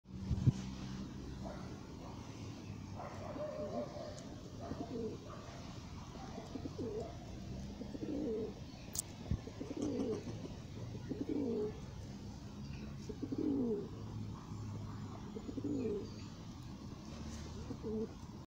Squirrel close up
Squirrel in glasgow botanic gardens taking a swipe trying to grab the camera end of the phone